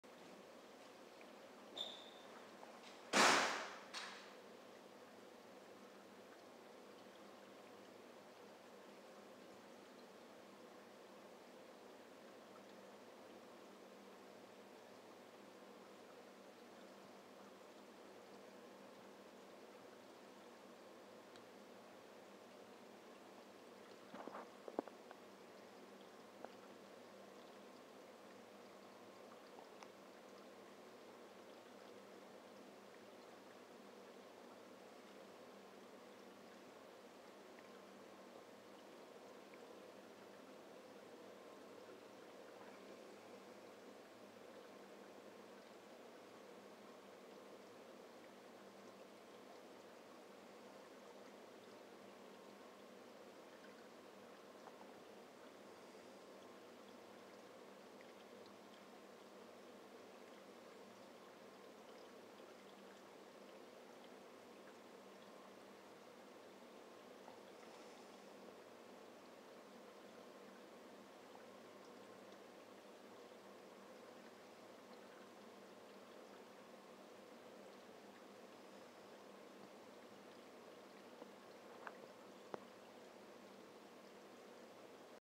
ORA Platinum Percula
A pair if Platinum Perculas contrasted with a Snowflake clown